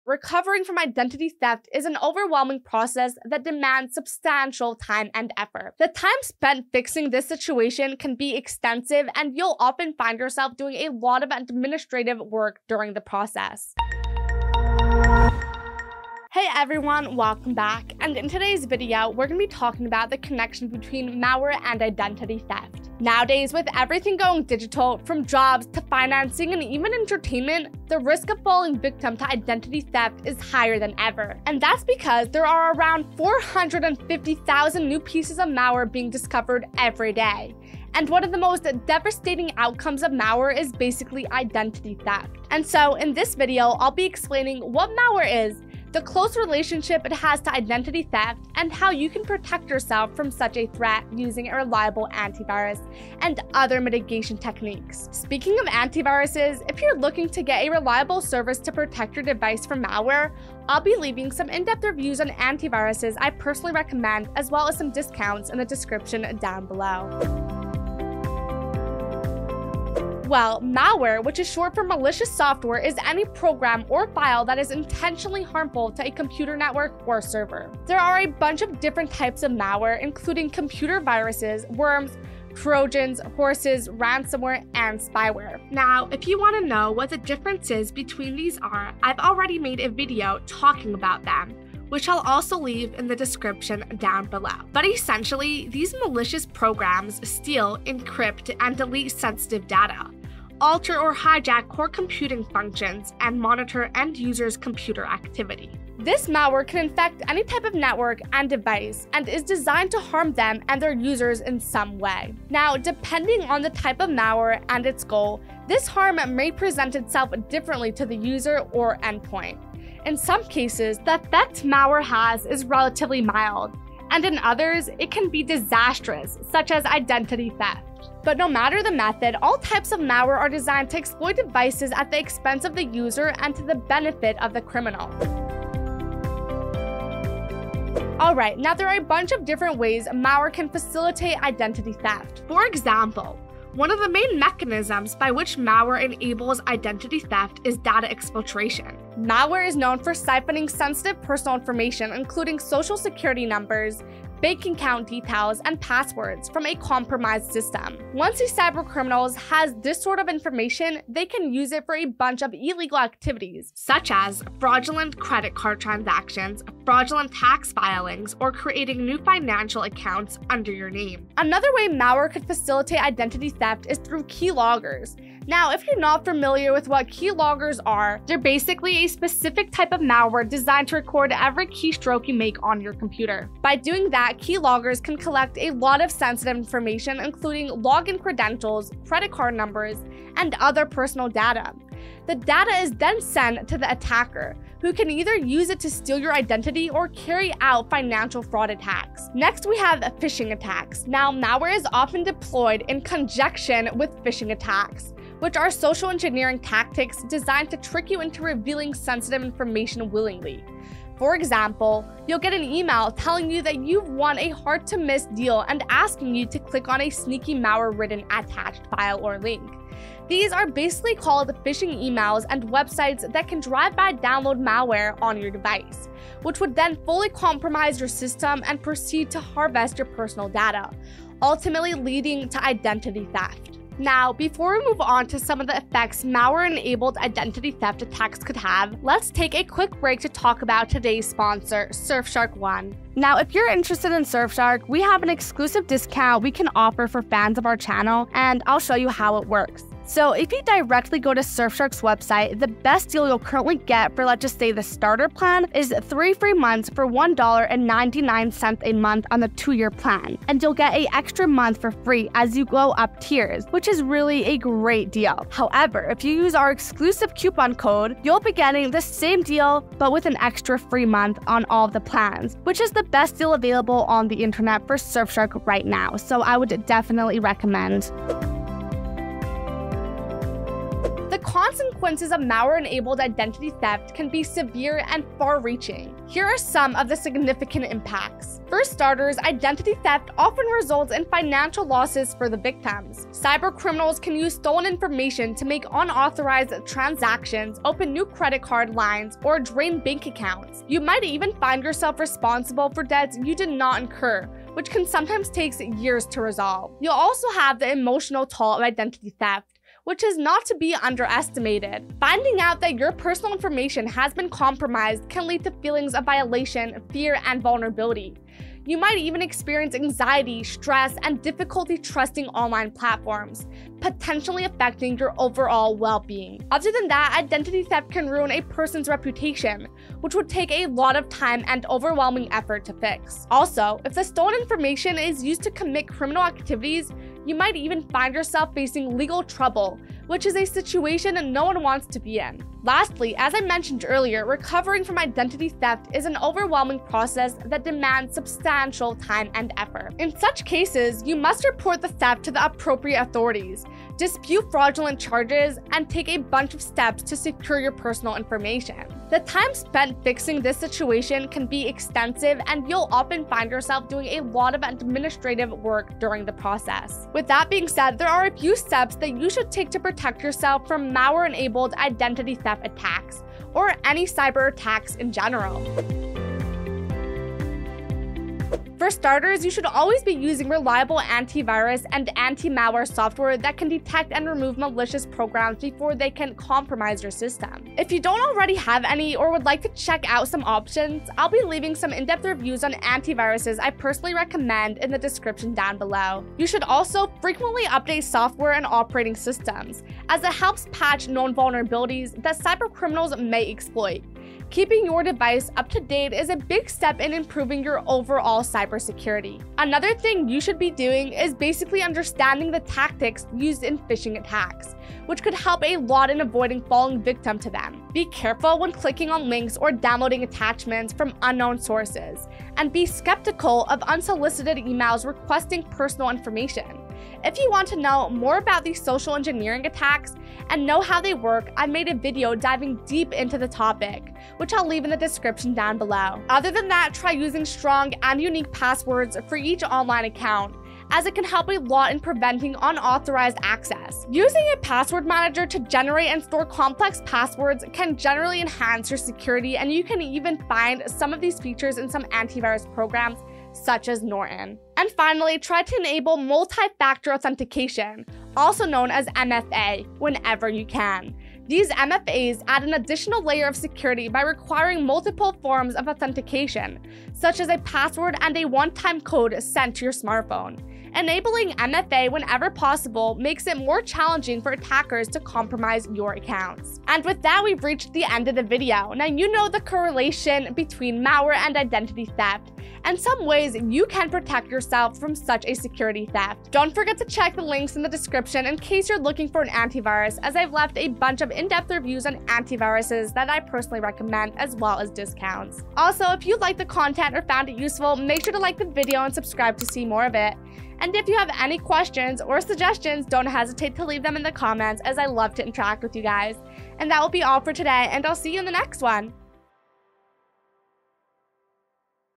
Malware's Connection to Identity Theft
Uncover the sinister link between malware and identity theft. Protect your digital life today.

💥EXCLUSIVE DISCOUNTS AUTOMATICALLY APPLIED TO ABOVE LINKS💥

Unveiling the World of Malware

Welcome back, everyone! Today, let's delve into the intricate connection between malware and identity theft in our increasingly digital world. With a staggering 450,000 new instances of malware discovered daily, the threat of identity theft looms large.

In this video, I'll demystify what malware is, ranging from viruses and worms to ransomware and spyware. To shield yourself from this menace, I'll also introduce reliable antivirus solutions and mitigation techniques. Check the video description for in-depth antivirus reviews and exclusive discounts.

Section 2: The Malicious Path to Identity Theft

Now, let's explore the ways malware serves as a catalyst for identity theft. Malware's primary tool for identity theft is data exfiltration, where personal information like Social Security numbers and passwords is siphoned from compromised systems.

Additionally, keyloggers, a specific malware type, record every keystroke, exposing sensitive data. Malware often collaborates with phishing attacks, tricking users into willingly revealing information. This section unveils the sinister mechanisms behind identity theft facilitated by malware.

Section 3: Mitigating Risks and Confronting Consequences

As we comprehend the threat, it's crucial to fathom the repercussions of malware-enabled identity theft. Victims face financial losses, emotional distress, damage to reputation, and potential legal ramifications. Recovering from such an attack demands time and effort, involving reporting to authorities and disputing fraudulent charges.

To fortify against these threats, employing reliable antivirus software, updating systems regularly, understanding phishing tactics, using strong passwords, and enabling Multi-Factor Authentication are pivotal steps. Check the description for antivirus reviews and discounts. Stay secure!

Hope you enjoyed my Malware's Connection To Identity Theft Video.